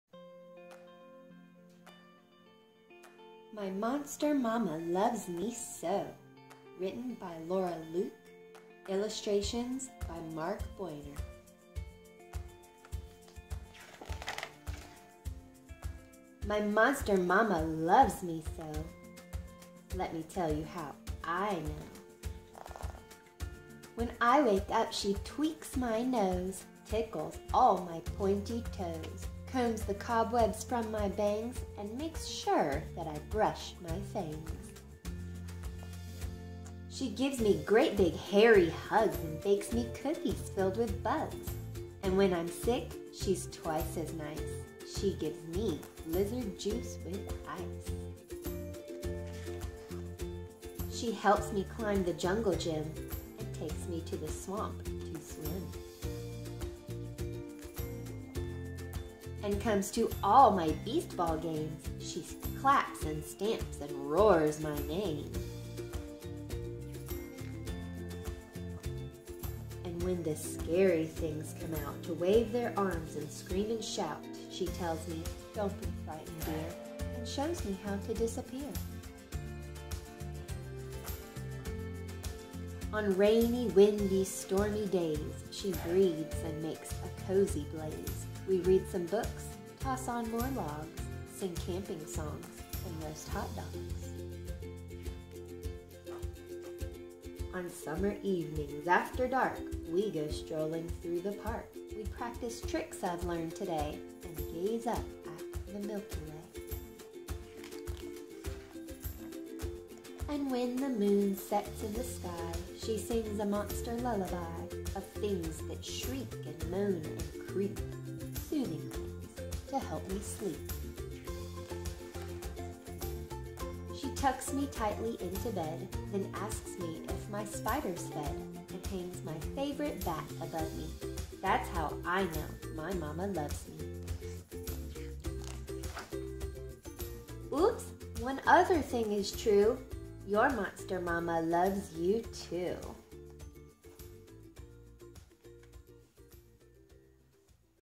My Monster Mama Loves Me So - Tiny Tales
This is a storybook read aloud of the book My Monster Mama Loves Me So written by Laura Leuk, illustrations by Mark Buehner. Perfect for naptime, bedtime, or anytime!